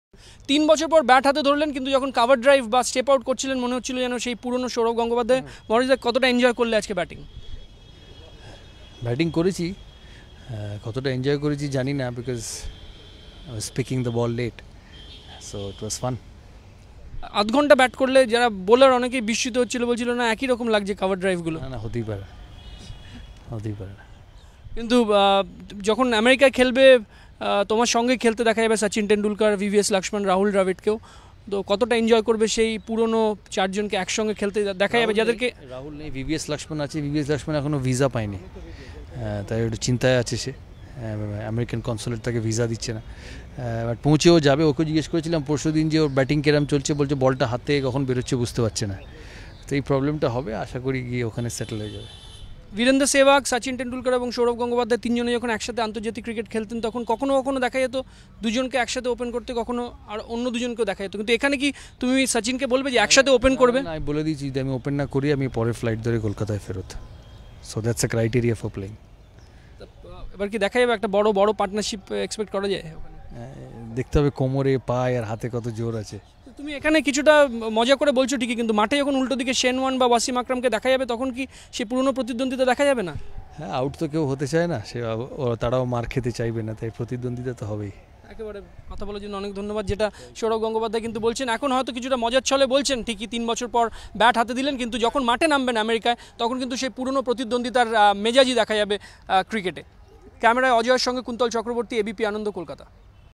Saurav Ganguly jokingly says, if he does n’t allowed to open in the exhibition match he
Saurav Ganguly jokingly says, if he does n’t allowed to open in the exhibition match he will come back in next flight